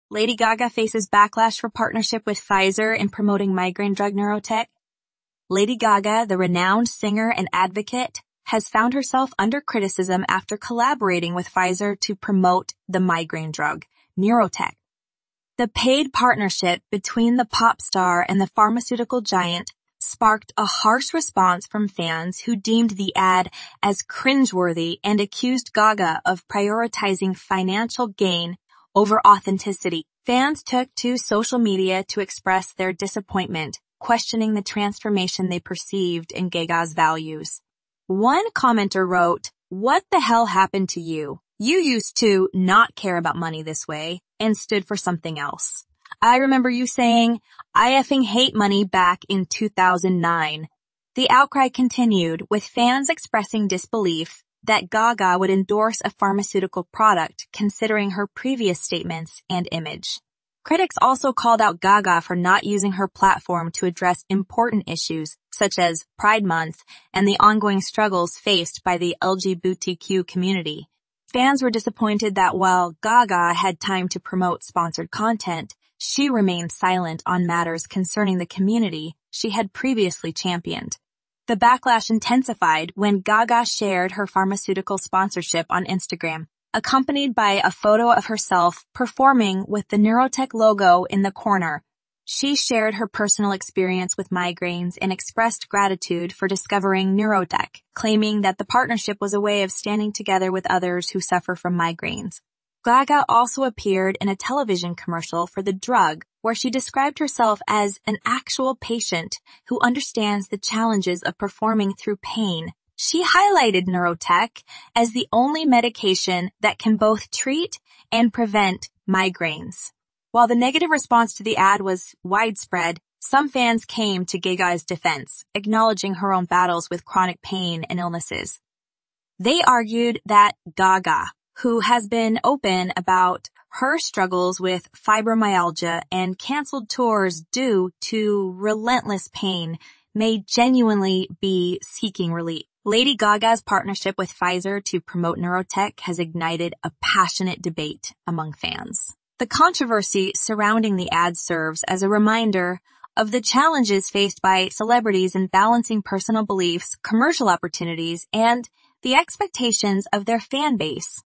The Backlash Storm: Lady Gaga's Neurotech Deal Divides Fans
Lady Gaga's Promotion of Pfizer's Migraine Drug Neurotech Draws Criticism

After teaming up with Pfizer to promote their migraine drug Neurotech, famous singer/activist Lady Gaga has come under fire for her involvement.

Many of Gaga's devoted followers took to social media to express their dismay and raise questions about a possible value shift on her part. What the hell happened to you?" asked one commenter. You used to care less about money and more about what you were standing for. You once said, "I effing hate money," and I remember that. Considering her previous statements and image, Gaga's fans were shocked to learn that she was endorsing a pharmaceutical product.

As part of their criticism, some pointed out that Gaga had ignored Pride Month and the continued struggles of the LGBTQ+ community. Many of Gaga's supporters were let down because she was able to devote time and energy to advertising sponsored content but remained silent on issues affecting the marginalized group she had previously championed.

The controversy surrounding Gaga's pharmaceutical sponsorship escalated after she posted an Instagram photo of herself performing alongside the Neurotech logo. She talked about having migraines herself and being grateful to have found Neurotech, saying that the partnership was a way to show solidarity with people who also suffer from migraines.

As a real patient who knows the struggles of performing through pain, Gaga also portrayed herself in a television commercial for the drug. She cited Neurotech as the only drug that treats and prevents migraines.

While many people were offended by the commercial, others rallied to Gaga's defense by pointing out that she has dealt with physical pain and illness herself. They contended that Gaga, who has spoken publicly about dealing with fibromyalgia and canceling tours due to constant pain, may be sincere in her search for relief.

Fans are very divided on whether or not they approve of Lady Gaga's collaboration with Pfizer to promote Neurotech. The uproar over the ad is a reminder of the difficulties celebrities face when trying to satisfy their fans while staying true to their own values and business interests.